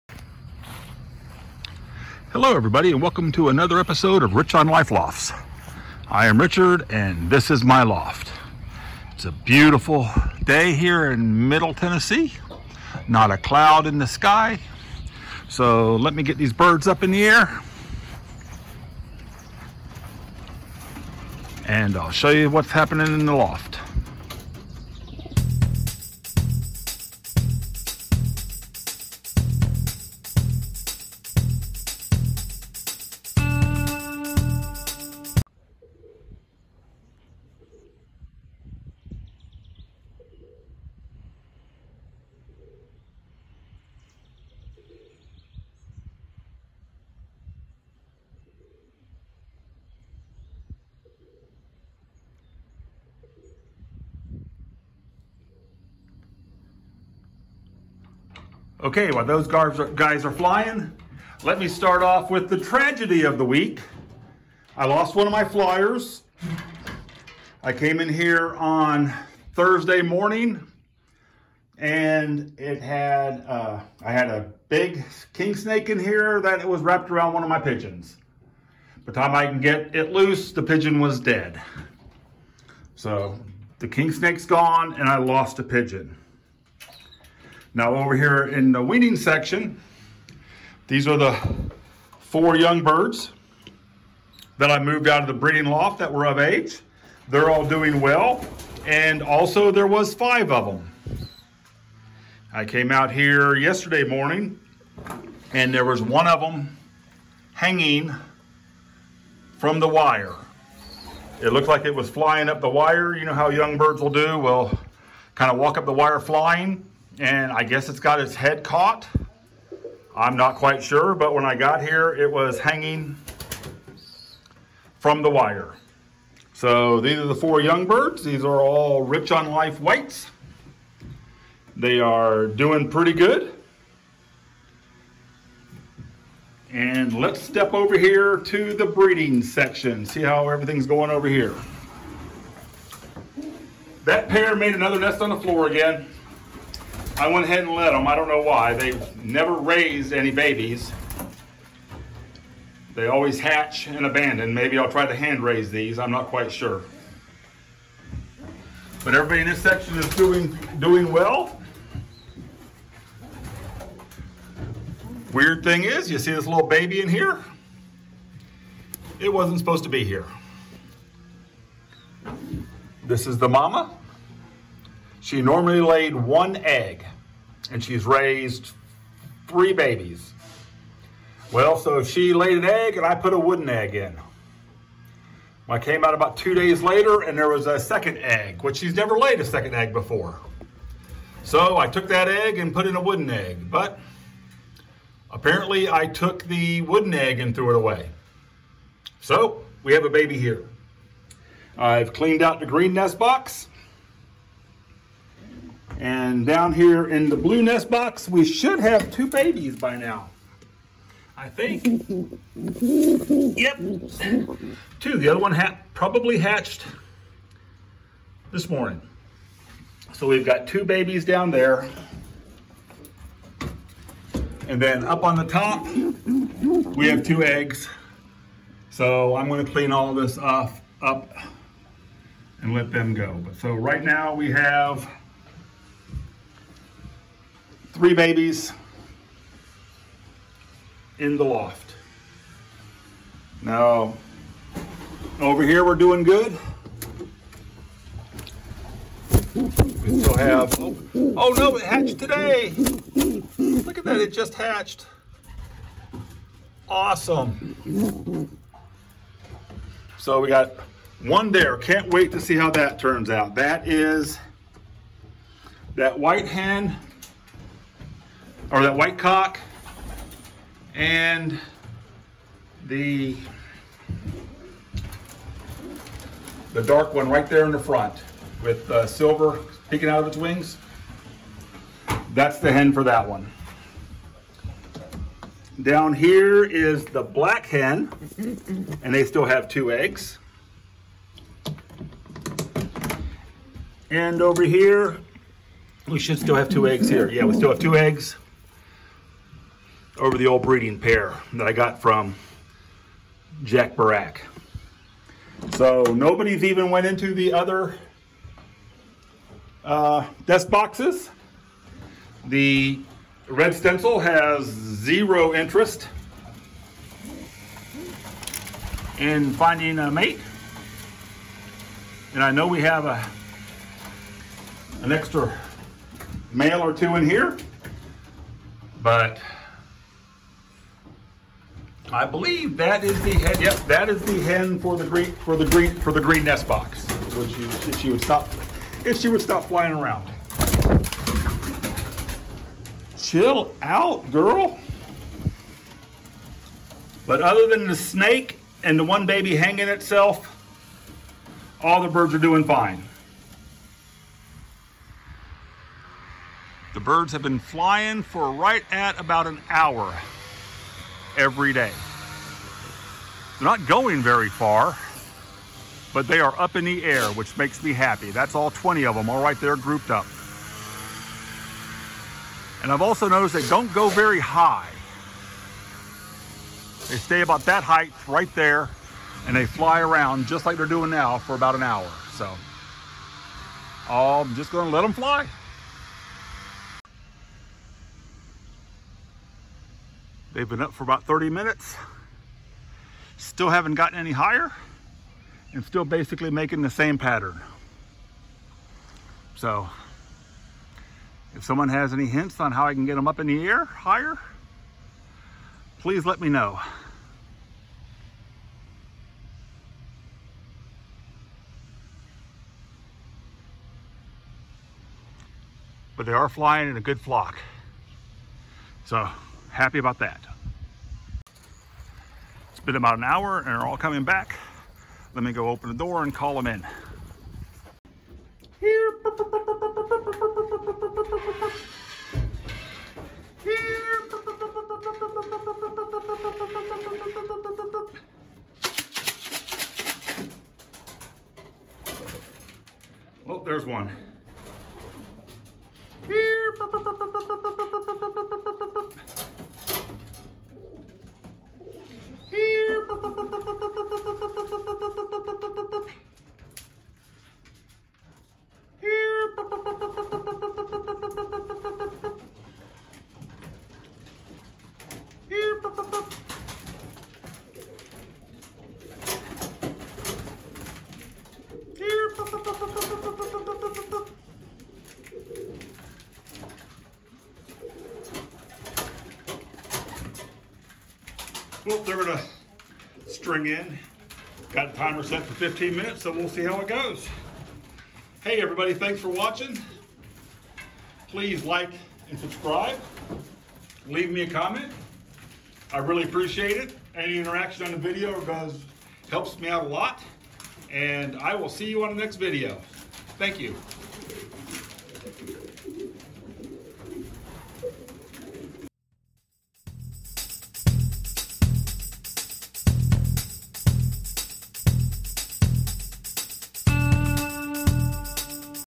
Snake Attack and an unexpected baby
Had a snake visit the loft and take a pigeon, also had an unexpected baby hatch.
Mail may be sent to:
Rich on Life Lofts

Water Bottle drinkers